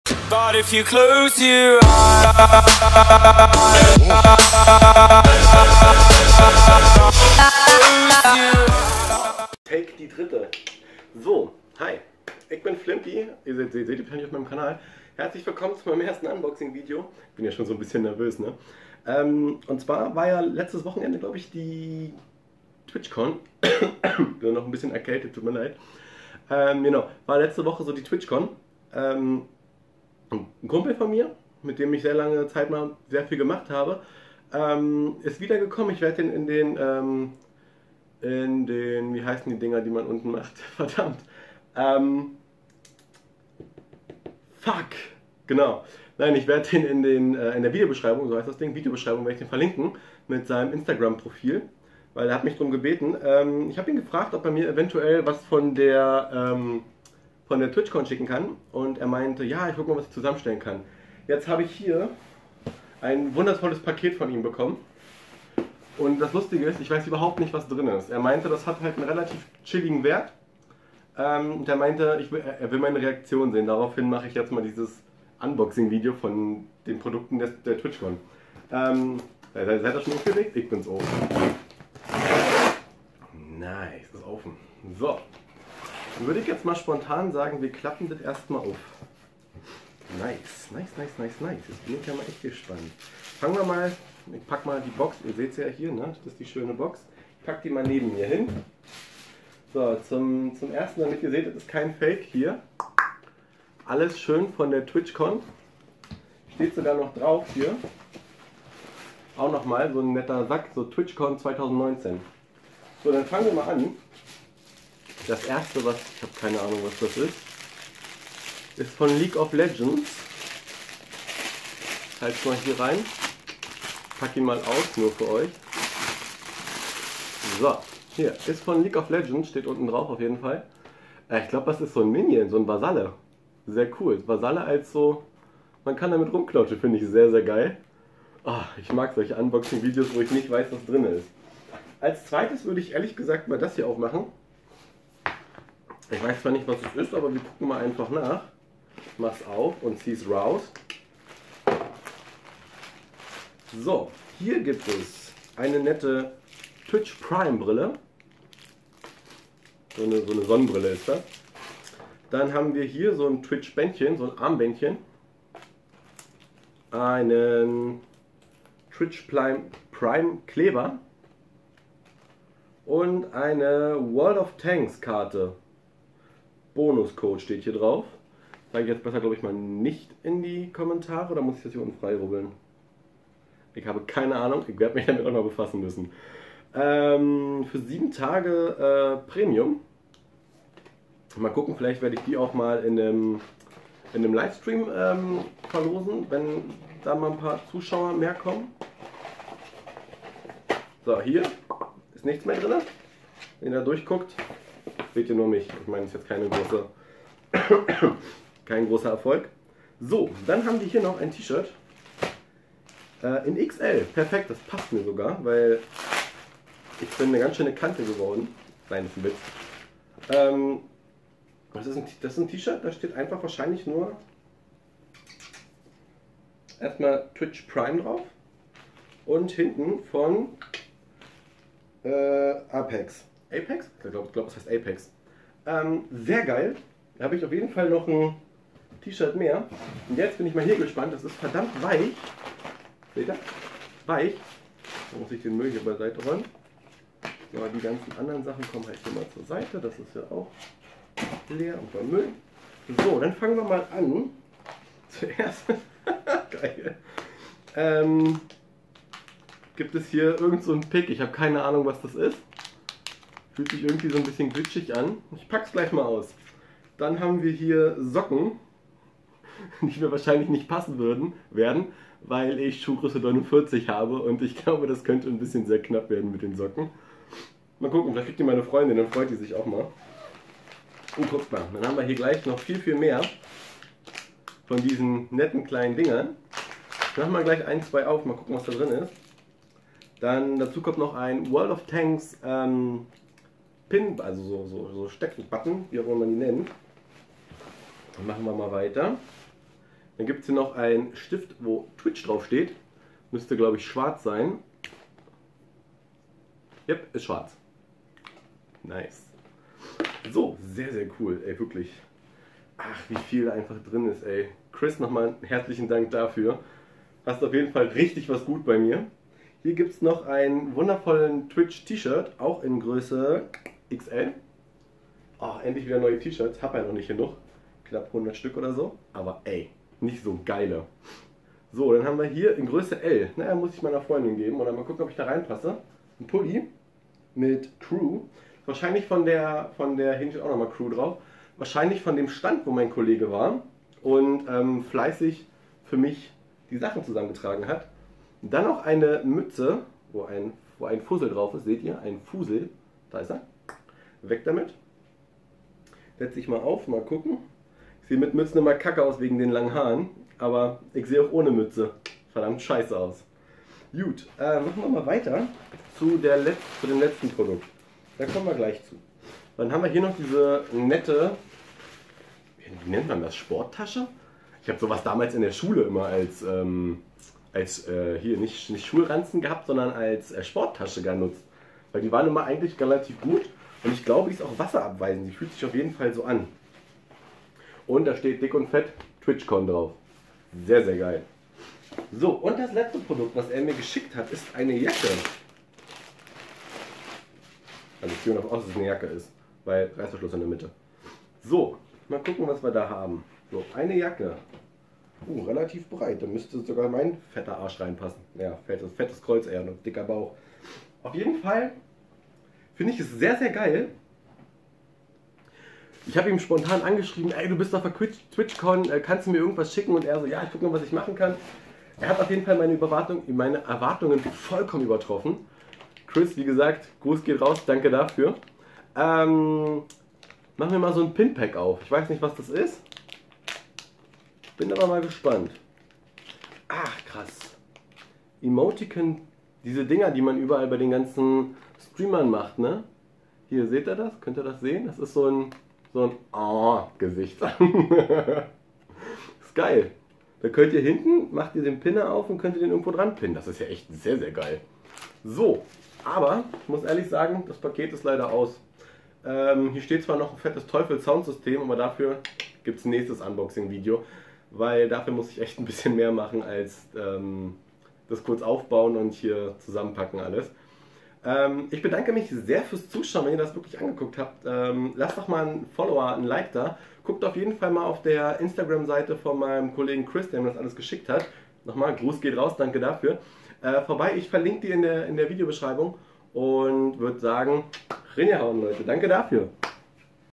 TwitchCon Paket Unboxing Was Bekomme ich da !!!!!!
TwitchCon Packet Unboxing

Tolle Sachen von der TwitchCon ich weis nicht was mich erwartet ^^
 wen ihr noch mehr davon sehen wolt schreibt mir doch in die Komentahre